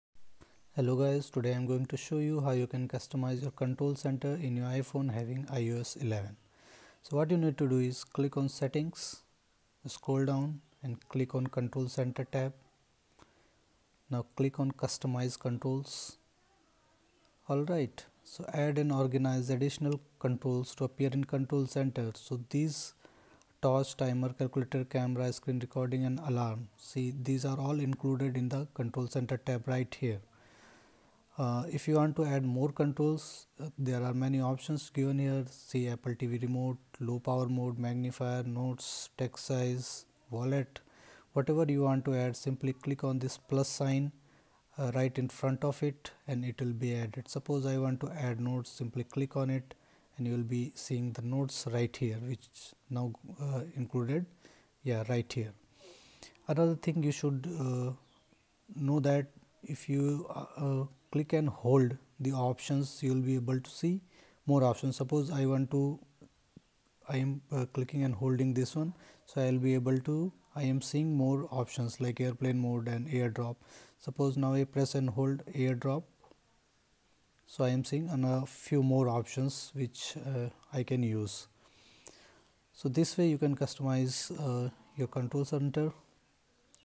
How to Customize Control Center in iPhone X/8/7/6
Learn how to add more items in control center of your iPhone X, iPhone 8, iPhone 7 having iOS 11. You can easily customize control center on your iPad, iPhone having iOS 11.